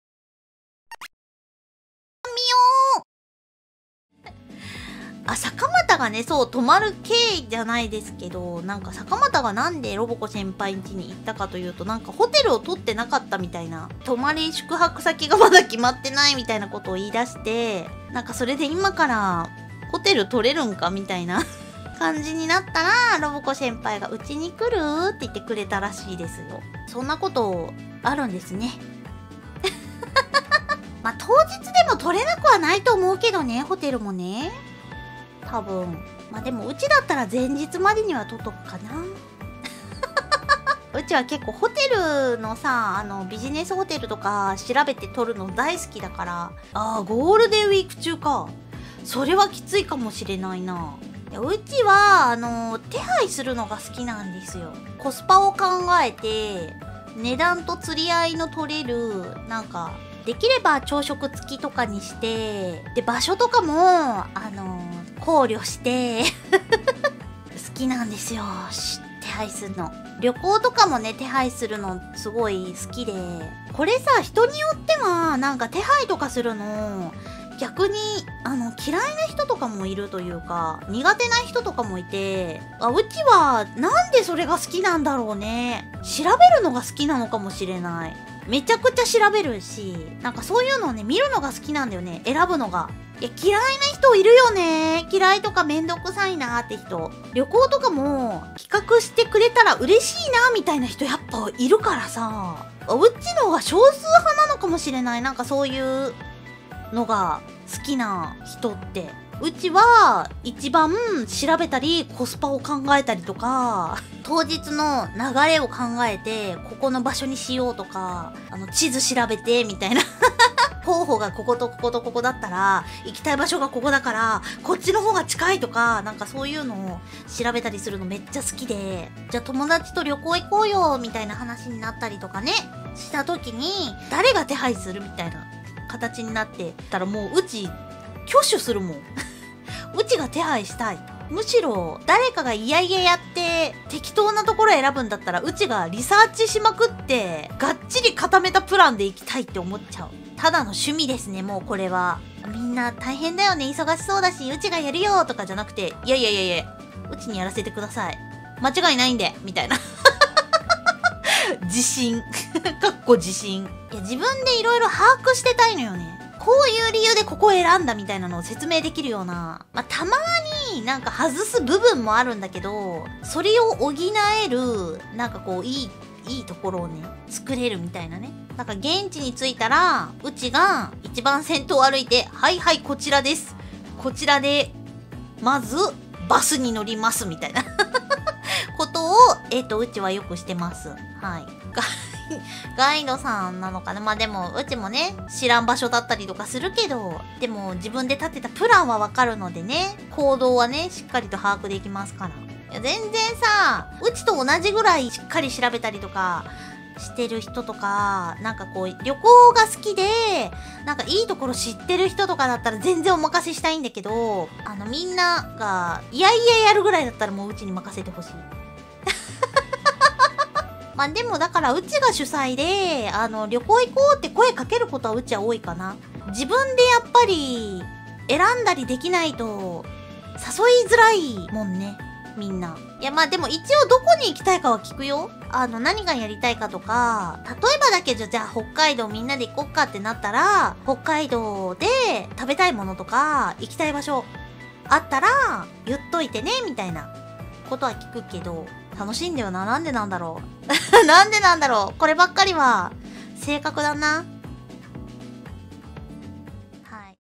[ 9 May 2022 ] Mio Explains Why She Is Usually The Organizer [ AsaMio / Eng Subs ]
Thus Mio Suba Miko Onsen Trip was born 🙏❤

I'm kinda the opposite 🤔 I prefer a no-plan trip; I'd know where I want to go, but I'd barely do any research on how to reach there (usually I travel for conventions, live concerts, etc) 🙏
I had a free day on one of my Japan trip, and I decided to do some random pilgrimage on the spot 😅

Special thanks to SynapseProperty for the amazing intro!